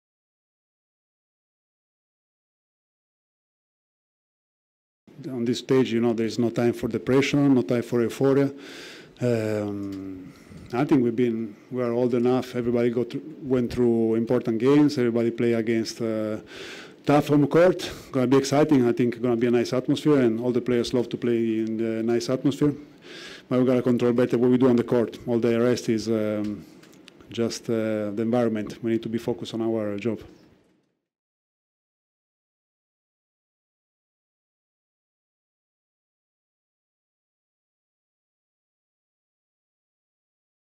No pressure for World No. 10 Italy facing tough Gilas home crowd
World No. 10 Italy is ready to make its stand against the Gilas Pilipinas home crowd when they meet for their final fixture of the group stage in the 2023 FIBA World Cup on Tuesday, Aug. 29, at the Smart Araneta Coliseum.

Coming off a loss against the Dominican Republic, Italy is looking to strengthen its hold of the No. 2 spot to see itself through the next round. (Video Courtesy of FIBA Media)